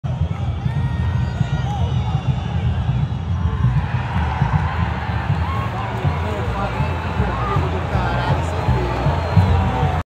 CORINTHIANS ELIMINADO DO PAULISTÃO NOS PÊNALTIS 😳😳
CORINTHIANS X ITUANO!